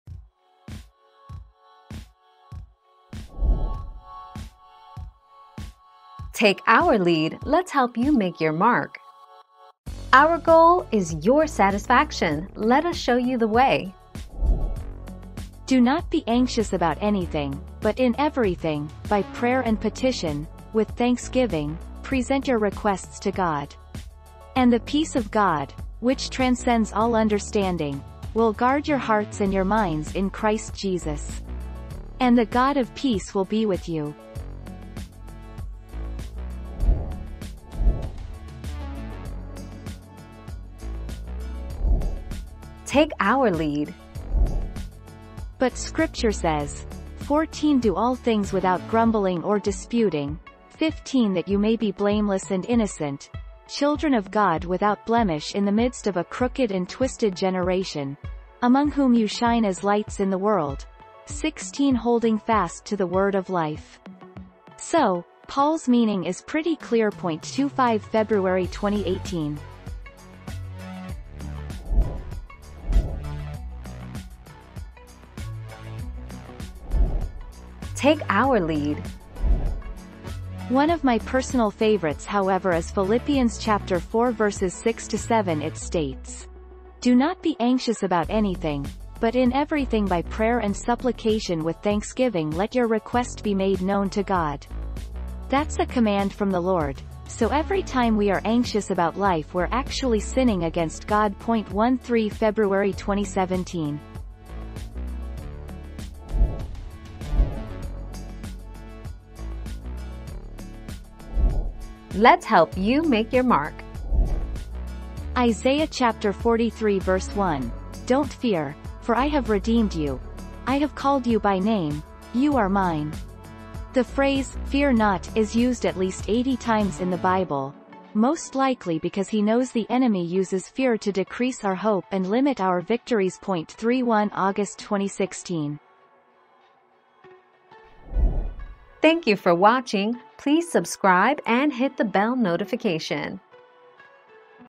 What does Don't worry about anything but in all NIV?
00:00 - What does Don't worry about anything but in all NIV?
00:38 - Does the Bible say anything without complaining?
01:13 - What does Philippians 4 6 & 7 mean?
01:47 - What Scripture says you have no fear?

Laura S. Harris (2021, April 12.) What does Don't worry about anything but in all NIV?
    AskAbout.video/articles/What-does-Don-t-worry-about-anything-but-in-all-NIV-235017